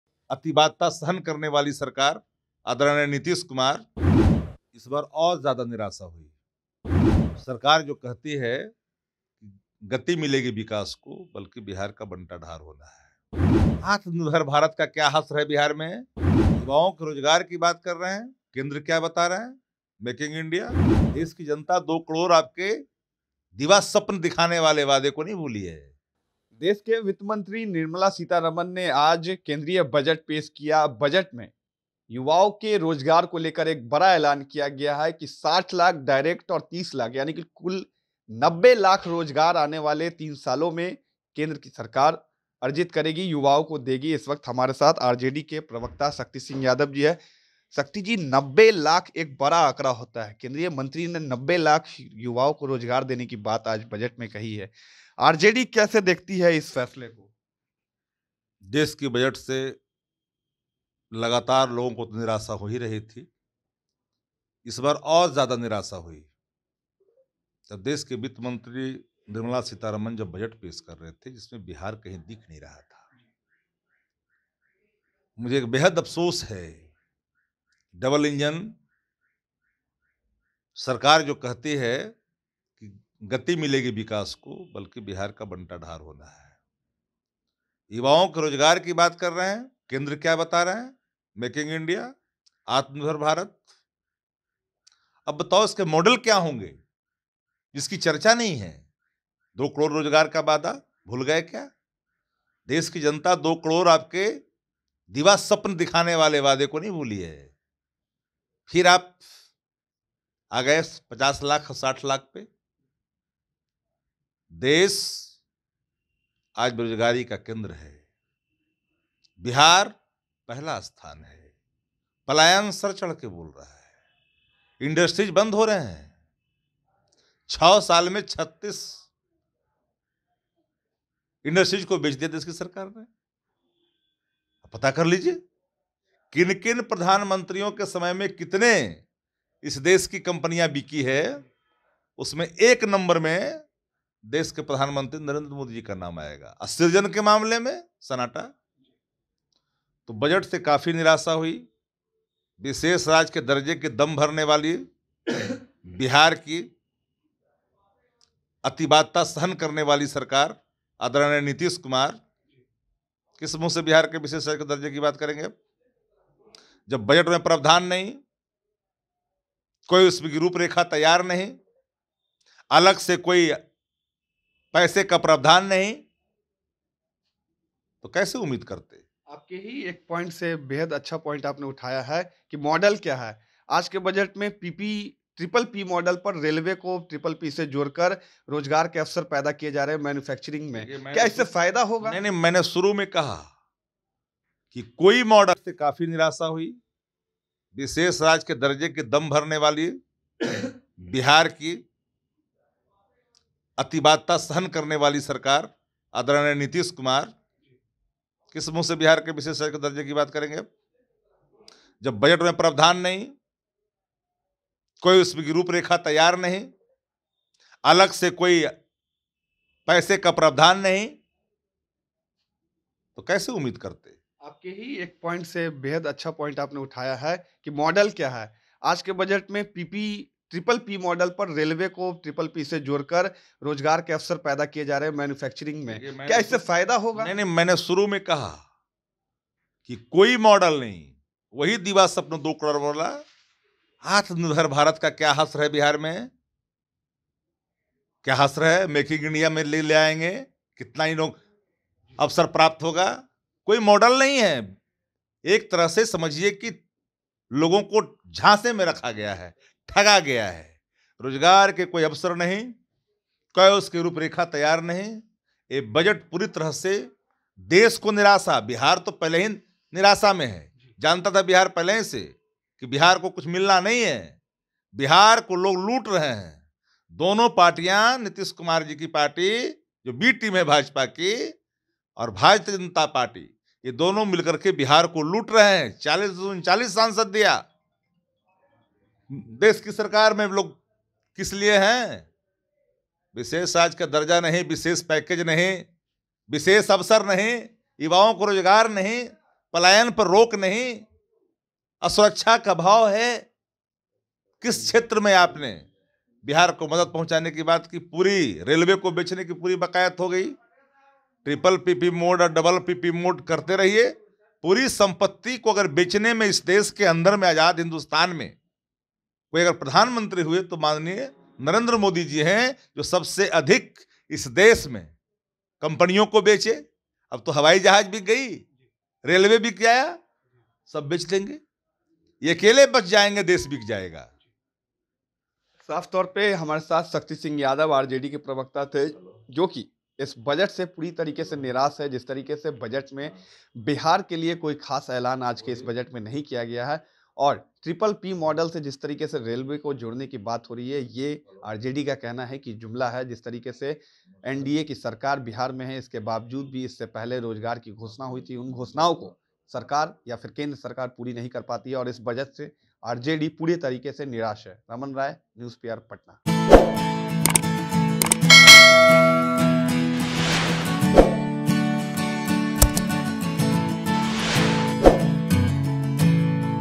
UNION BUDGET : RJD नेता शक्ति सिंह यादव बोले, केन्द्रीय बजट से लोग ठगा गये |NEWS PR
UNION BUDGET : RJD नेता शक्ति सिंह यादव बोले, केन्द्रीय बजट से लोग ठगा गये |